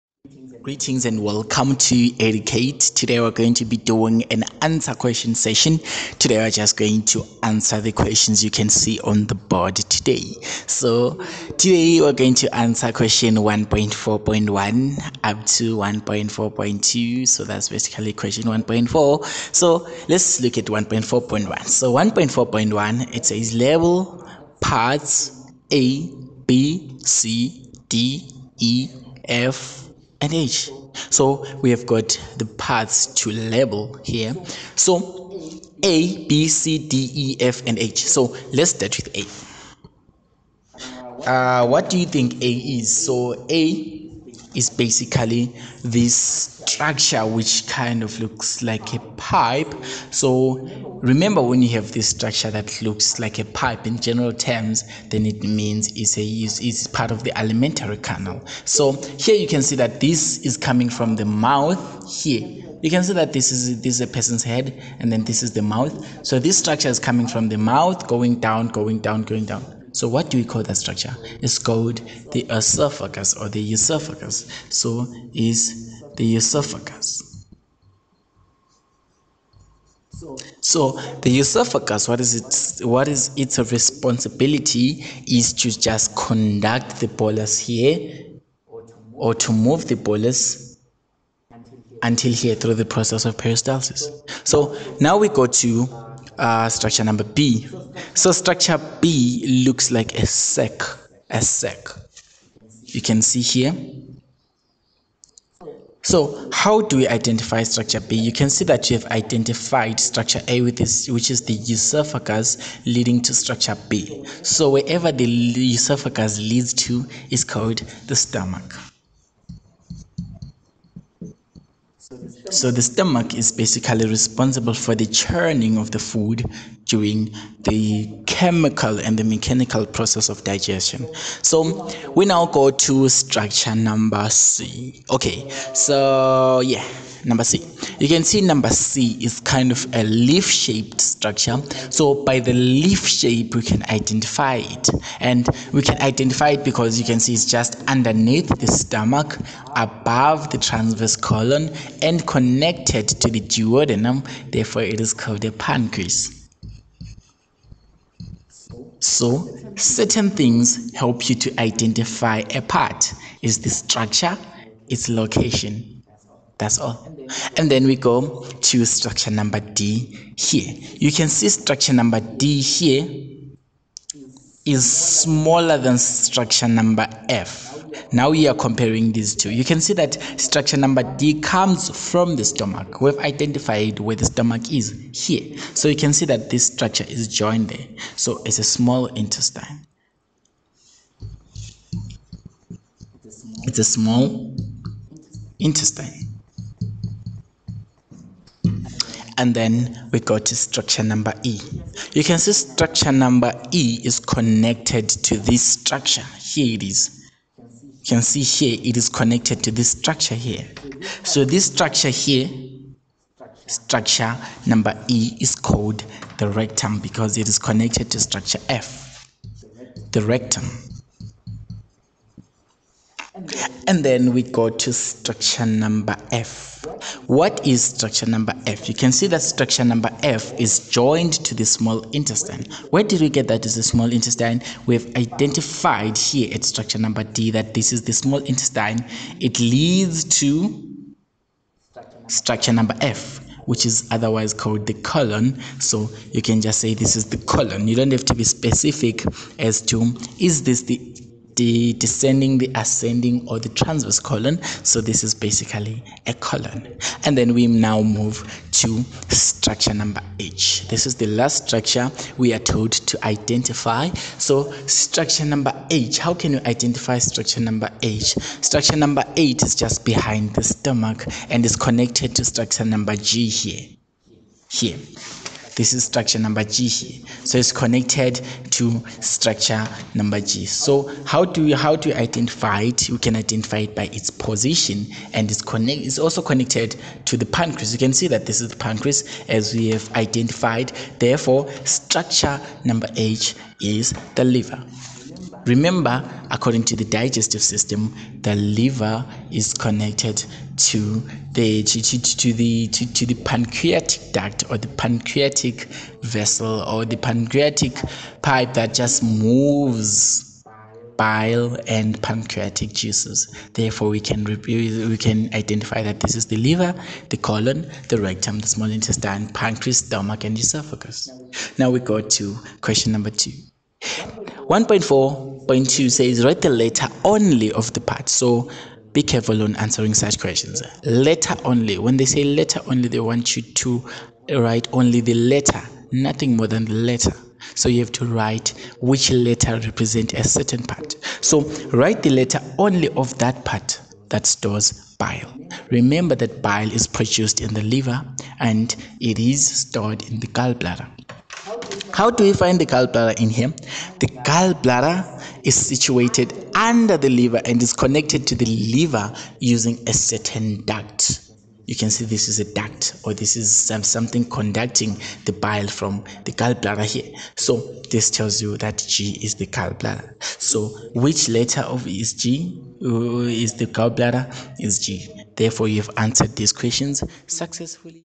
Digestive System (activity) - Grade 11 Life Sciences
Hey there Today we're going to be answering the questions based on what we learnt in animal nutrition.

Timings:
00:00 Introduction
00:23 Parts of the digestive system